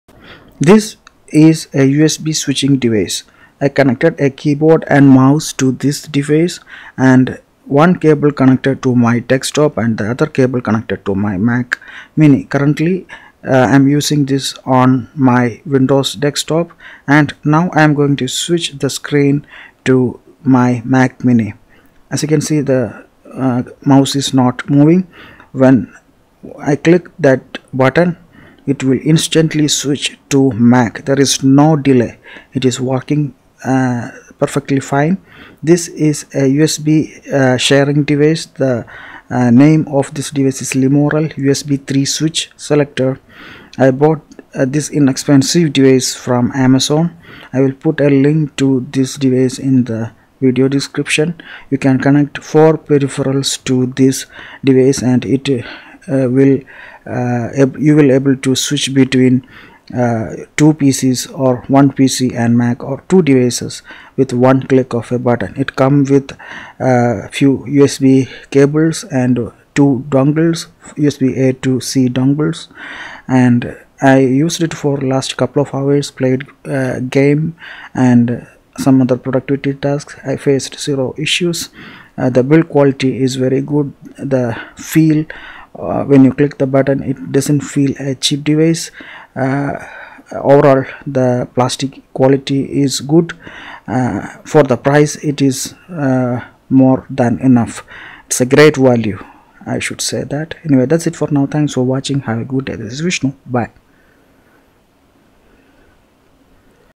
Lemorele USB 3.0 Switch: Share 4 USB Devices with 2 Computers with a Single Click.
Amazon India
Amazon USA
Lemorele USB 3.0 Switch Selector 2 Computers Sharing 4 USB Devices 4-Port USB Peripheral KVM Switcher Box for PC, Mouse, Keyboard, Printer, Scanner with 2 USB Cables, Compatible Windows/Mac/Linux
【Multi-device sharing】 The Lemorele USB 3.0 switch can share 4 USB 3.0 peripherals between 2 computers, and can also connect printers, scanners, mice, keyboards, USB drives, hard drives, etc.
【One-click switching】No need to frequently plug and unplug cables or set up complicated network sharing software, you can switch back and forth between 2 computers in a second with just click of a button, and the LED light shows the computer that is being activated.
【Improve work efficiency】Using Lemorele switch selector can greatly improve work efficiency, USB interface supports 5Gb/s data transfer rate, can perfectly cooperate with high-bandwidth USB 3.0 devices, such as external hard disks, high-resolution webcams, cameras, video surveillance cameras, digital cameras, multi-channel audio, etc., and backward compatible with USB 2.0 and USB 1.1 devices.
【Excellent compatibility】This USB 3.0 switch has good compatibility, no driver, works for Windows/Mac/Linux, etc., just plug and play.
【What you get】1 x USB 3.0 Switch, 1 x power cable, 2 x USB-A to USB-A cables (𝐍𝐨𝐭𝐞： it is not recommended to use a charger with higher voltage output than 5V2A, it may affect the performance of the product)